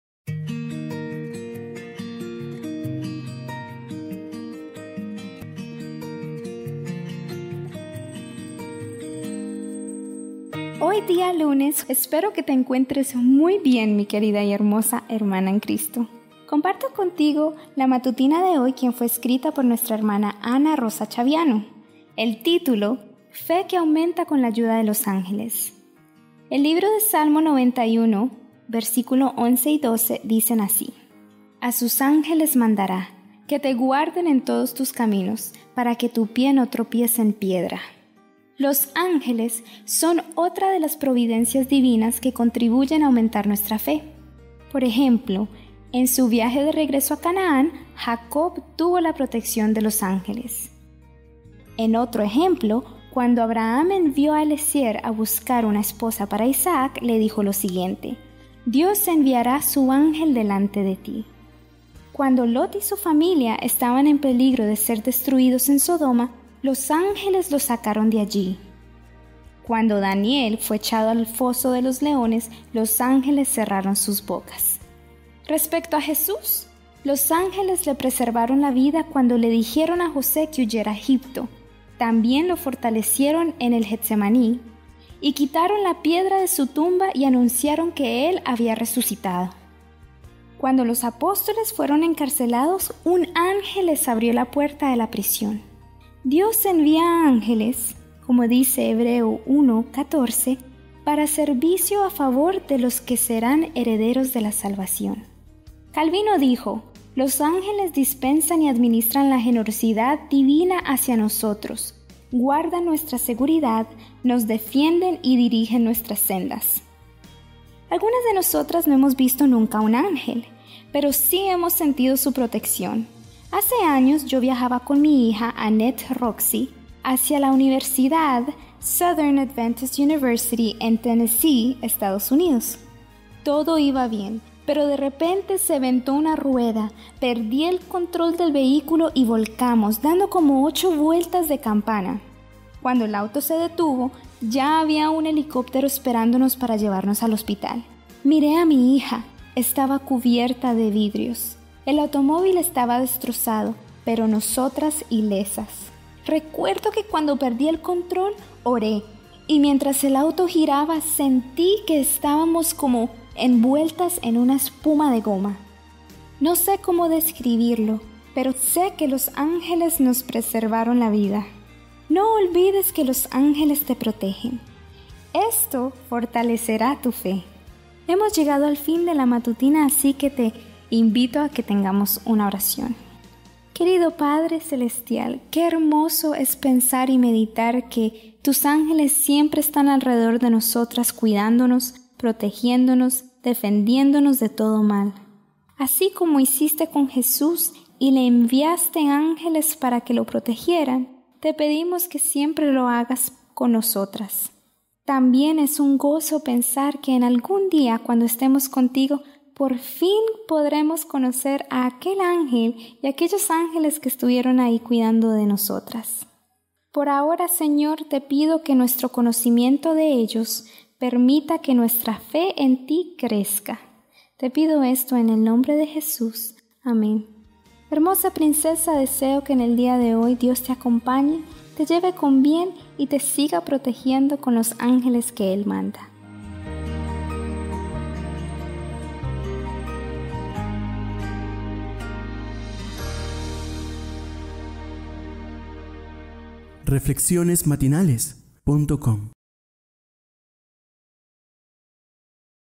Matutina de Damas: Lunes 8 de Junio 2015
FE QUE AUMENTA CON LA AYUDA DE LOS ÁNGELES

A sus ángeles mandará... que te guarden en todos tus caminos... Para que tu pie no tropiece en piedra. Salmo 91:11, 12

Los ángeles son otra de las providencias divinas que contribuyen a aumentar nuestra fe. En su viaje de regreso a Canaán, Jacob tuvo la protección de los ángeles (Gén. 32:1, 2). Cuando Abraham envió a Eliezer a buscar una esposa para Isaac, le dijo: “[Dios] enviará su ángel delante de ti” (Gén. 24:7). Cuando Lot y su familia estaban en peligro de ser destruidos en Sodoma, los ángeles los sacaron de allí (Gén. 19:15-17). Cuando Daniel fue echado al foso de los leones, los ángeles cerraron sus bocas (Dan. 6:22). Respecto a Jesús, los ángeles le preservaron la vida cuando le dijeron a José que huyera a Egipto (Mat. 2:13); lo fortalecieron en el Getsemaní (Luc. 22:40-43); quitaron la piedra de su tumba y anunciaron que él había resucitado (Mat. 28:2, 5, 6). Cuando los apóstoles fueron encarcelados, un ángel les abrió las puertas de la prisión (Hech. 5:19, 20).

Dios envía ángeles “para servicio a favor de los que serán herederos de la salvación” (Heb. 1:14). Calvino dijo: “Los ángeles dispensan y administran la generosidad divina hacia nosotros; guardan nuestra seguridad, nos defienden y dirigen nuestras sendas”.*

Algunas no hemos visto nunca un ángel, pero sí hemos sentido su protección. Hace años, yo viajaba con mi hija, Annette Roxie, hacia la Southern Adventist University, en Tennessee, Estados Unidos. Todo iba bien pero, de repente, se reventó una rueda, perdí el control del vehículo, y volcamos, dando como ocho vueltas de campana. Cuando el auto se detuvo, ya había un helicóptero esperándonos para llevarnos al hospital. Miré a mi hija. Estaba cubierta de vidrios. El automóvil estaba destrozado, pero nosotras ilesas. Recuerdo que, cuando perdí el control, oré y, mientras el auto giraba, sentí que estábamos “como envueltas en una espuma de goma”. Los ángeles nos preservaron la vida.

No olvides que los ángeles te protegen. Esto fortalecerá tu fe.

Ana Rosa Chaviano.

* Juan Calvino, Institutes of the Christian Religion, t. 1.

Narrado por: Juliana Arias
Lecturas Devocionales para Damas 2015
Jardines del Alma
Por: Diane de Aguirre